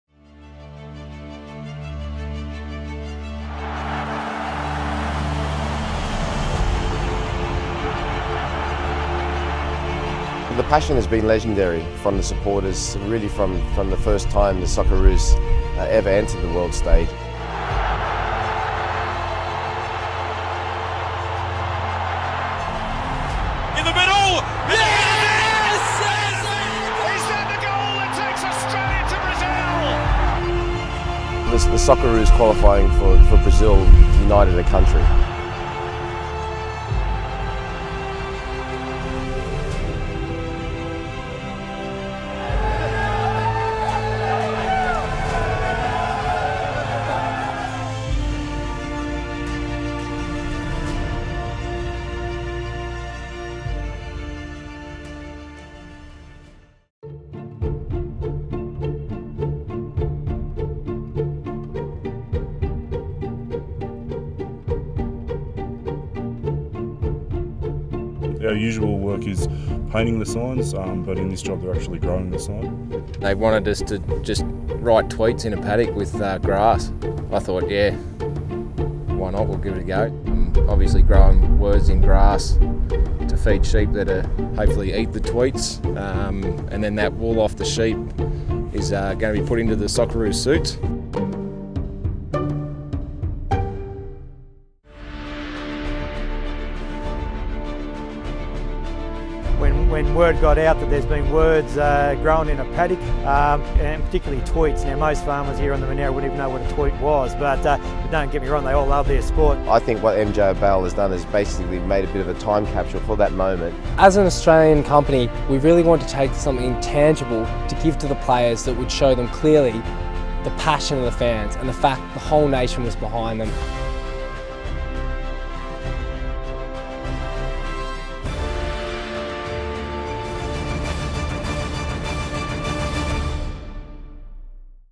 Paddock of Pride: Grazed on Australian Sporting Passion
MJ Bale - the Official Clothier to the Socceroos, decided to weave the fans' passion into the team suits.